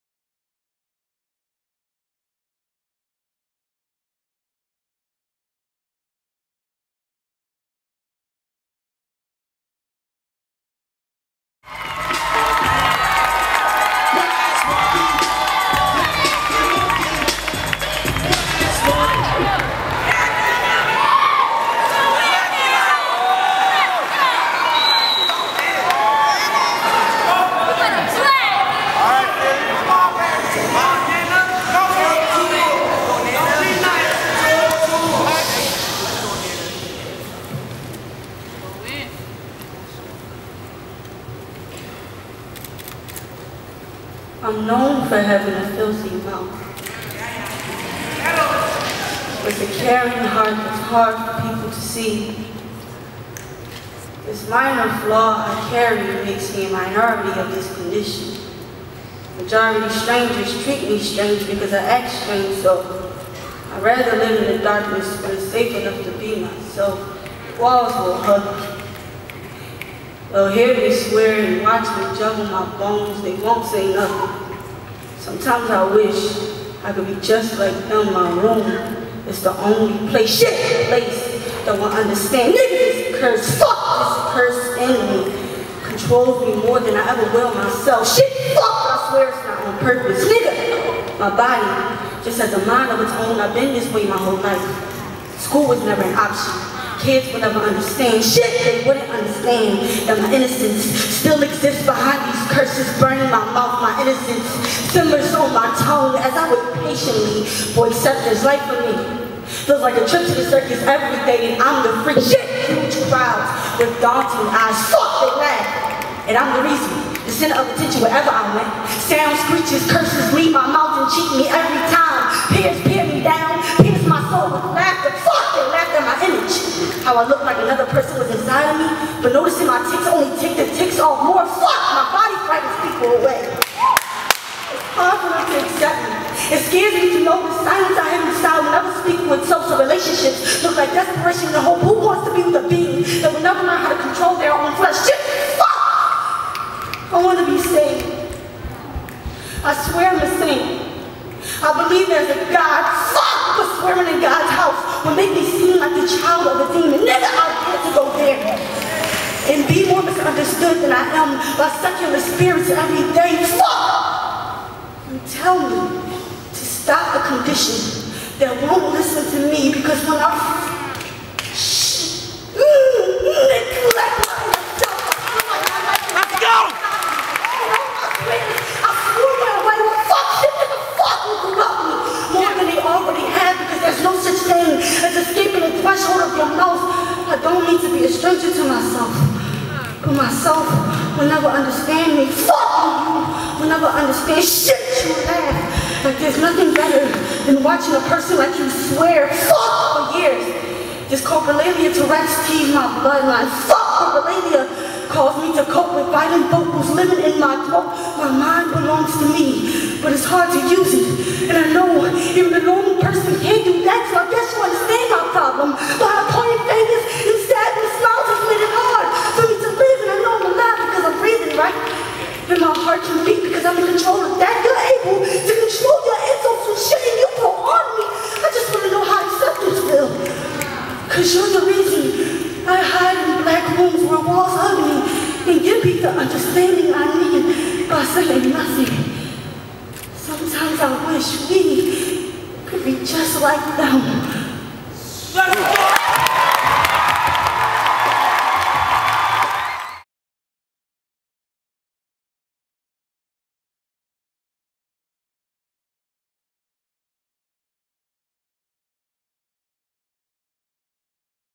2012 - Brave New Voices (Finals) - Philadelphia Round #3
Brave New Voices 2012 at the fabulous Fox Theatre in downtown Oakland. Listen to the love. Feel the meaning in your heart. Witness the Word!